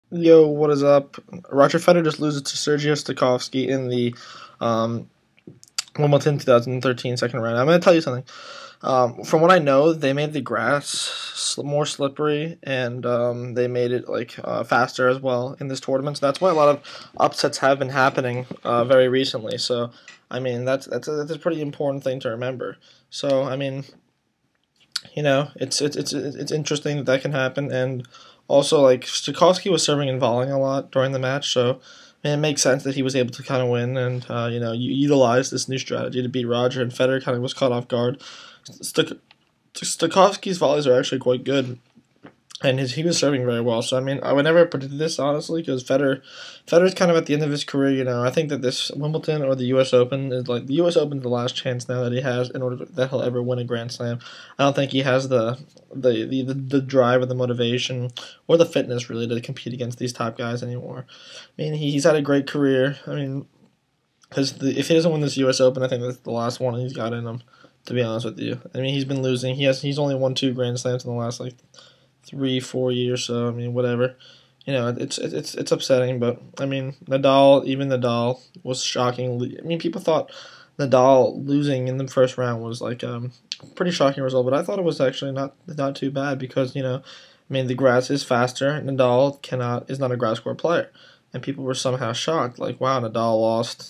Roger Federer vs. Sergiy Stakhovsky WImbledon 2013 2nd Round
Roger Federer loses to Sergiy Stakhovsky in the Wimbledon 2013 2nd Round. Federer has not lost before the quarterfinals of a grand slam in many years.